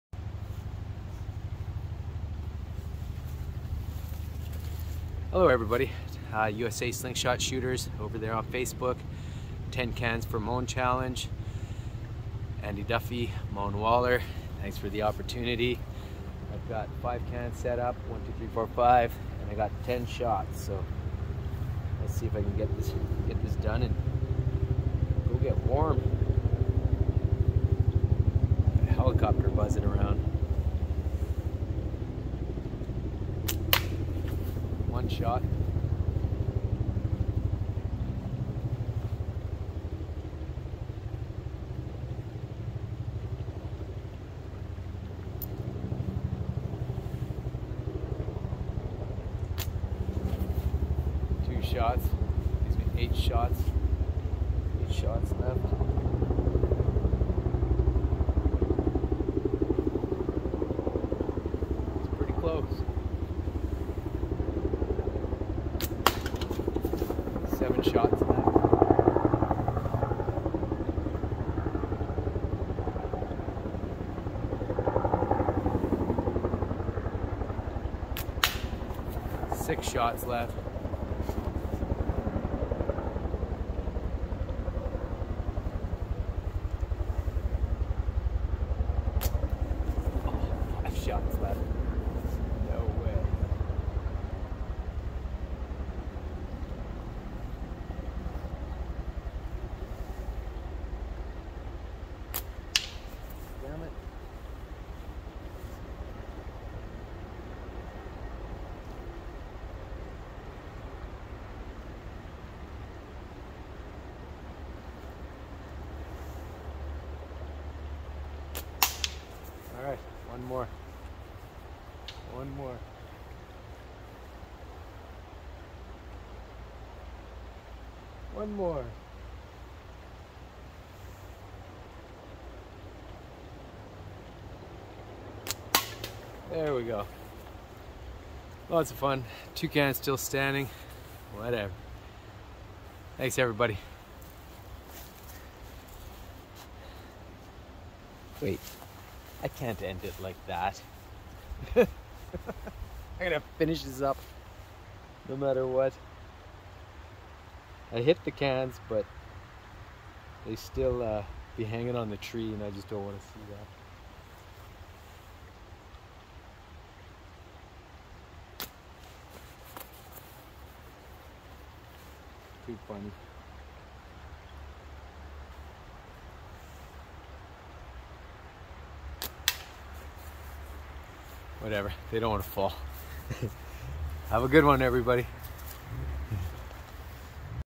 10Shots 10Meters 5Cans
USA Slingshot Shooters
Mone Waller Andy Duffey Can Challenge
10 Shots 10 Meters 5 Cans
Having fun with a slingshot challenge
Opportunity to win one of Mone Wallers Custom Handmade Slingshots
To good to pass up!